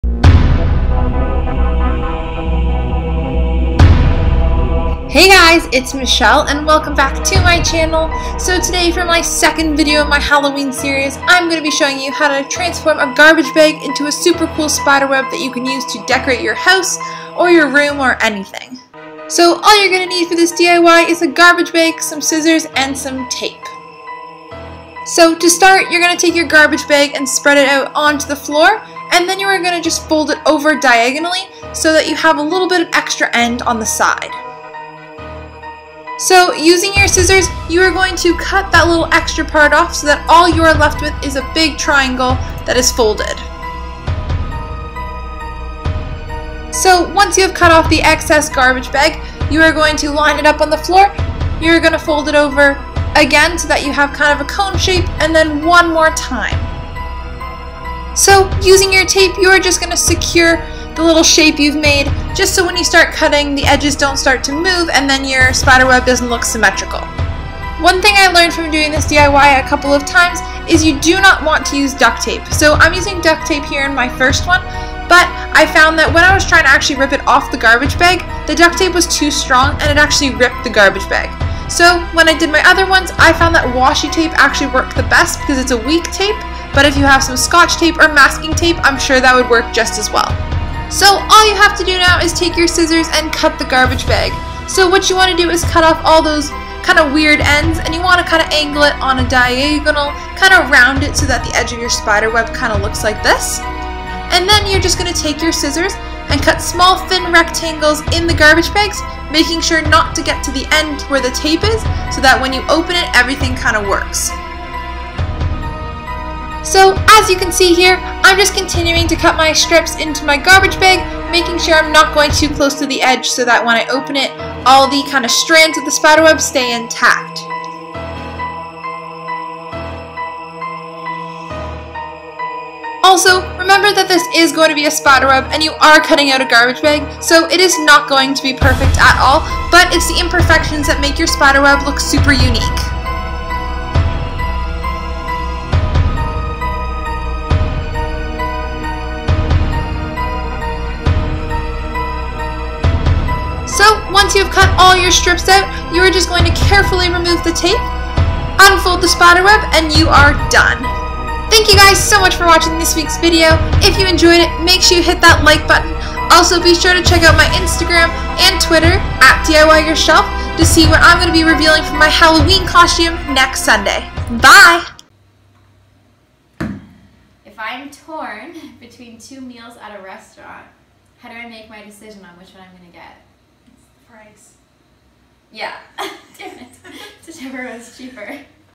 DIY Garbage Bag Spider Web
hey guys!
for the second video in my halloween series, I decided to do a super easy garbage bag spider web! These are super easy to make and can be use to decorate both the outside and inside of your house for halloween!